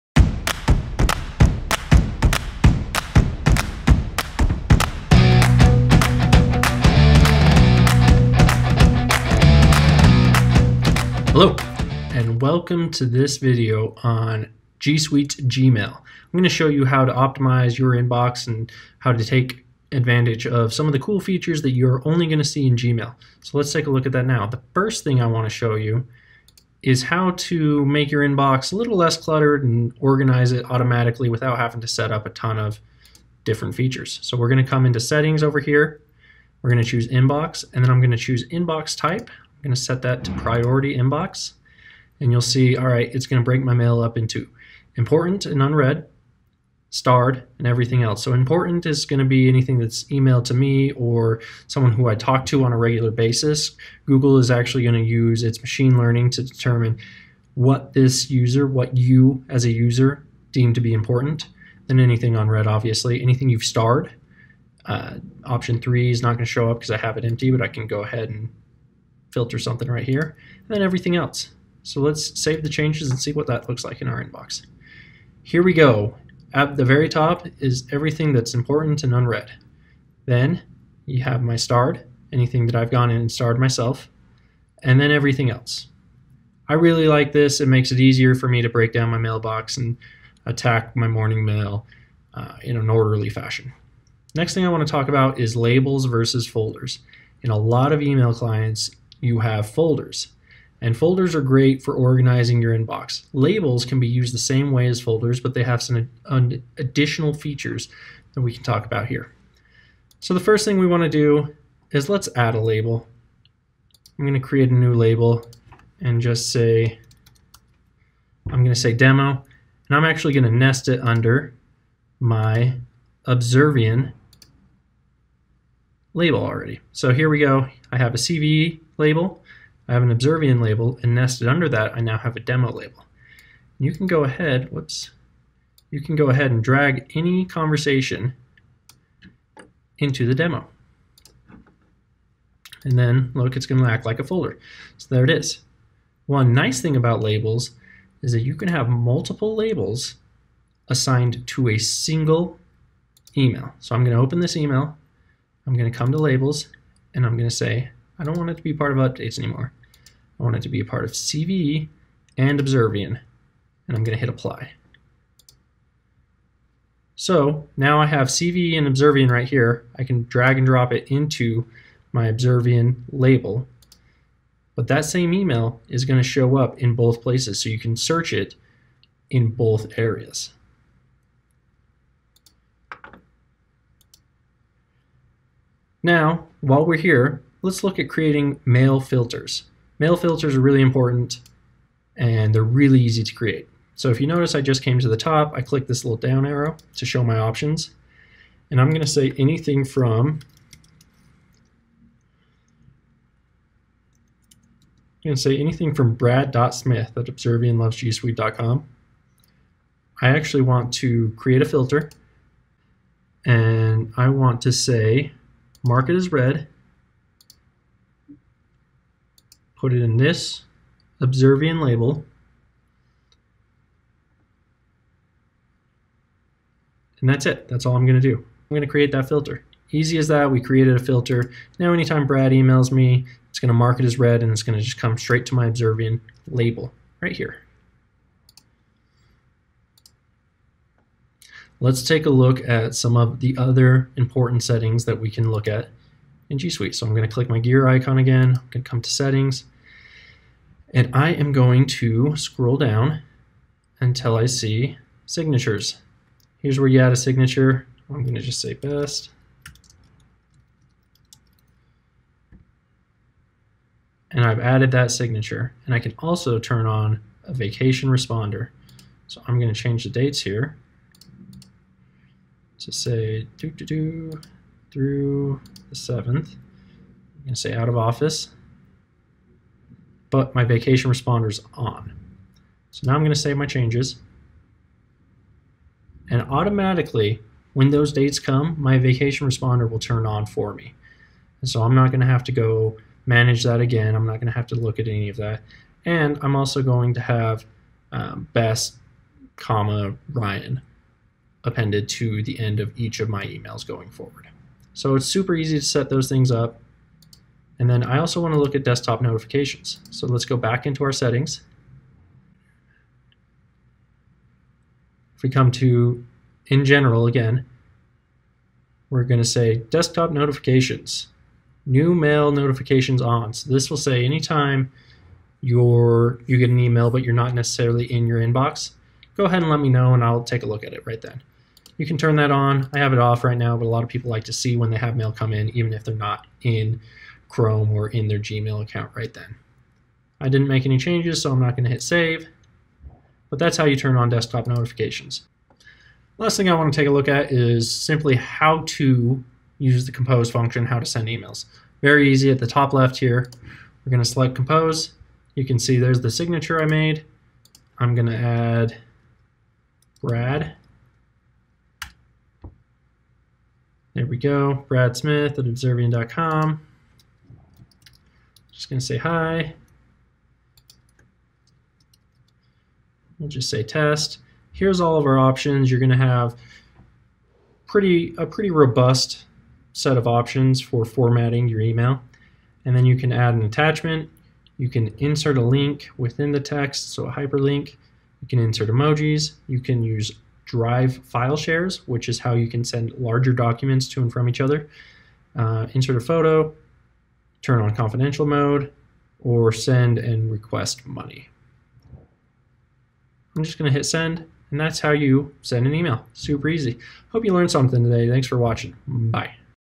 Gmail Crash Course
Learn the best tips and tricks to using Gmail. Set filters, signatures and learn about labels in under ten minutes!